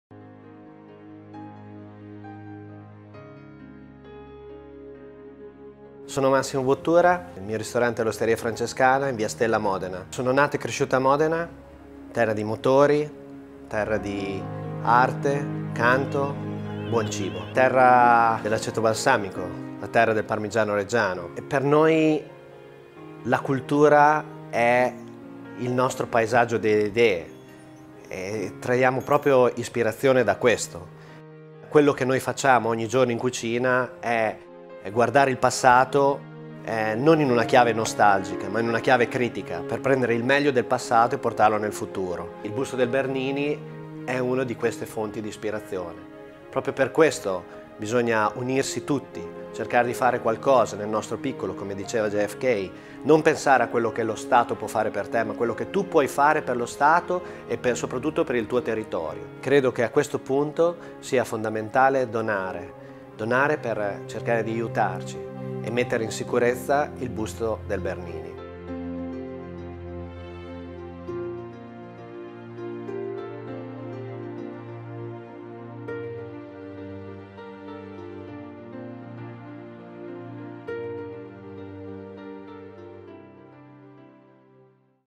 Massimo Bottura per For Italy
Massimo Bottura - chef e proprietario dell'Osteria Francescana, ristorante tre stelle Michelin a Modena - sostiene la campagna di crowdfunding promossa da For Italy per la realizzazione di un innovativo piedistallo antisismico che consentirà di esporre in totale sicurezza il busto di Francesco I d'Este, capolavoro di Gian Lorenzo Bernini, orgoglio e simbolo della Galleria Estense di Modena.